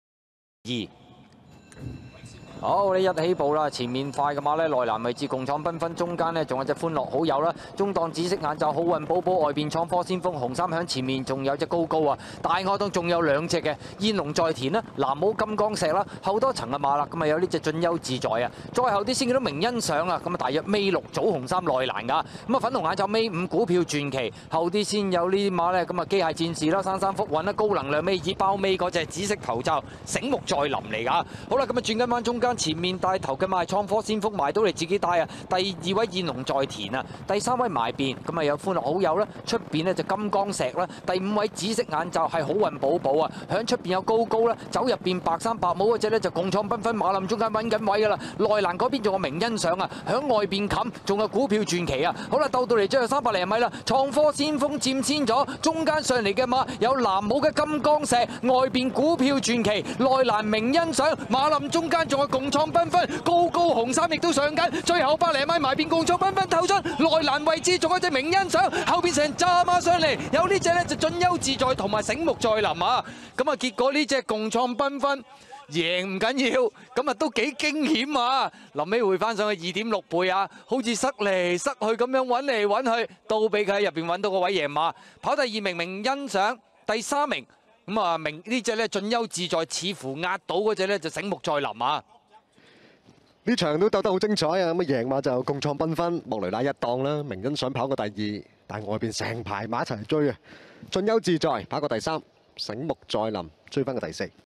20/09/2020 賽事場次(042) 沙田第四班(石籬讓賽)-1200米賽事重溫, 共創繽紛操好而出,末段險遇阻塞, 稍有空位交出反應, 及時發力終點險勝
1  共創繽紛 莫雷拉
2  明欣賞         周俊樂
3  進優自在 田泰安
4  醒目再臨 波健士

按加入就可加入做會員